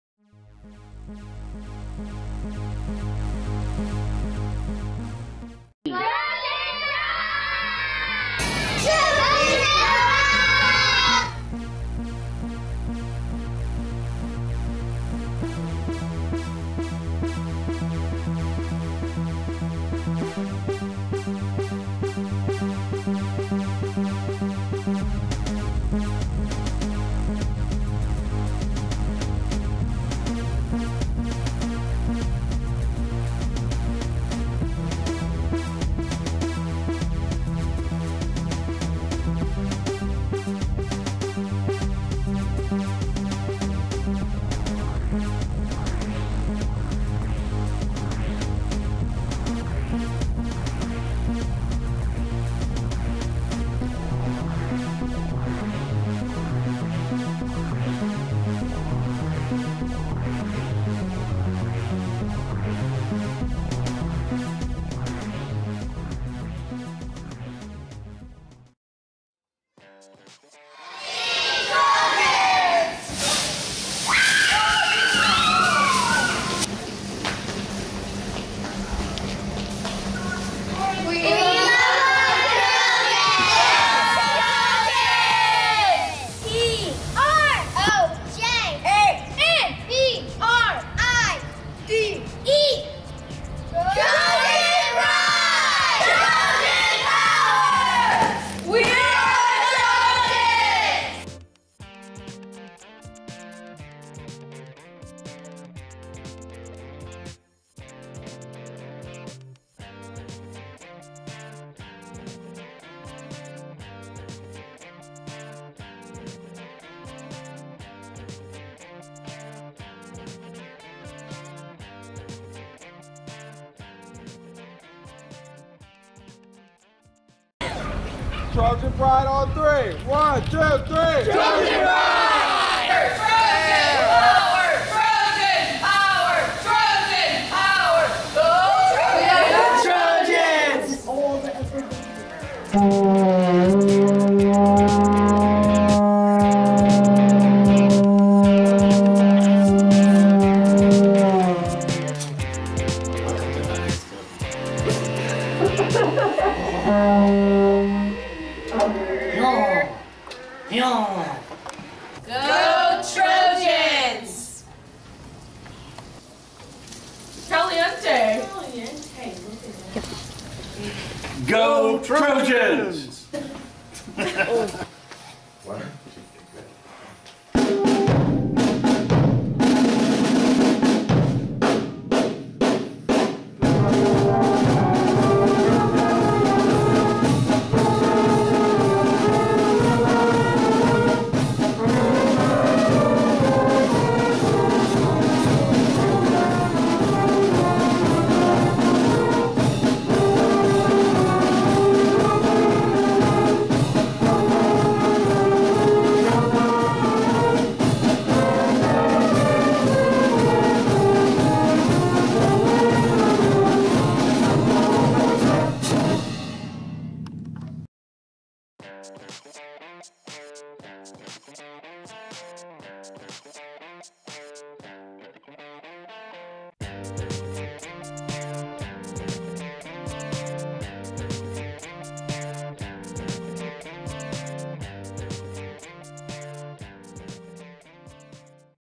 Pride
This is a video created and directed by the 7th Grade keyboarding class during the spring of 2016.